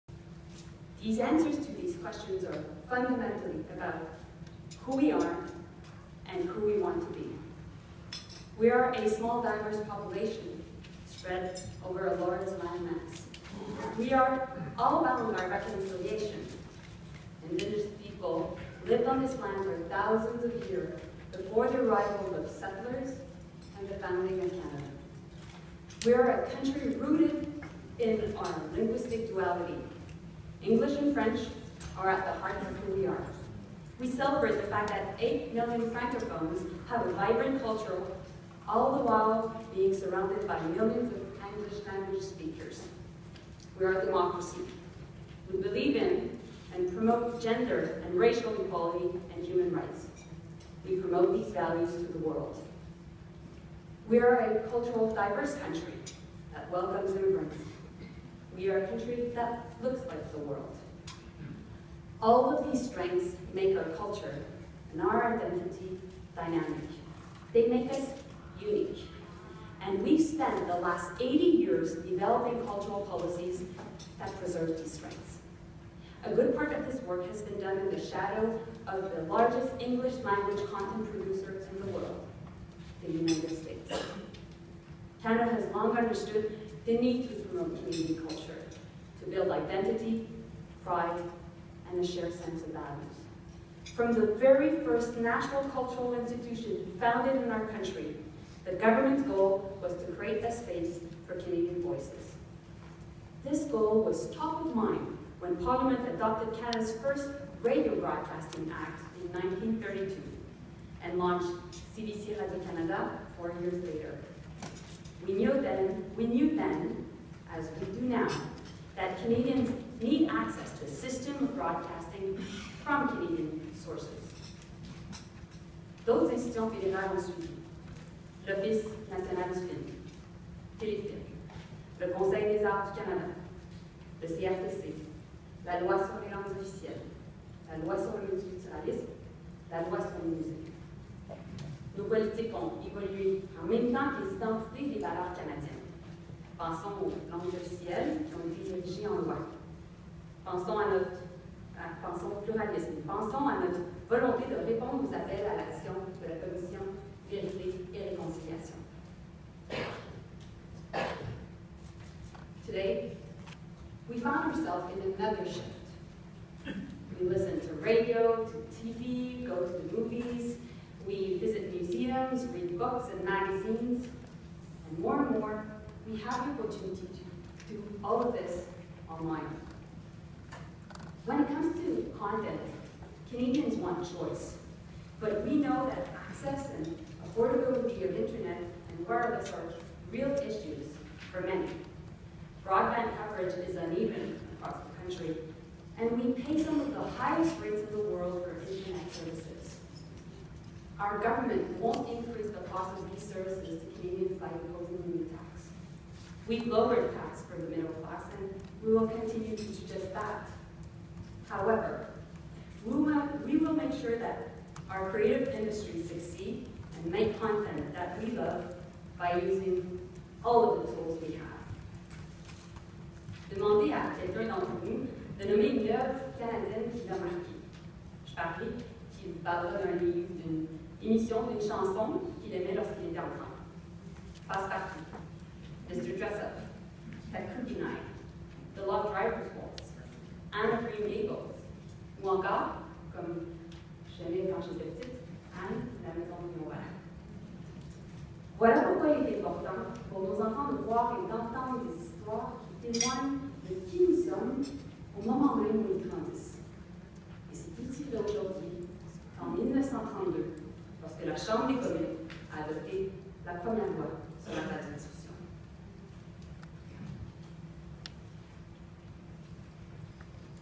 Melanie Joly on new Broadcasting Policy (Video #1)
Canadian Heritage Minister Melanie Joly announces new approach to broadcasting in the digital age